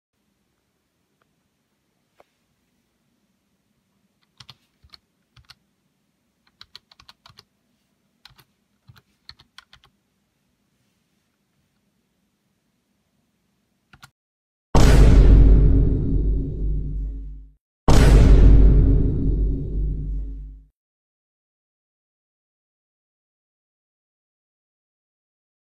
Codename: Beach House (Short Version)
This is the short version of the teaser for my upcoming short film which will be released in 2022.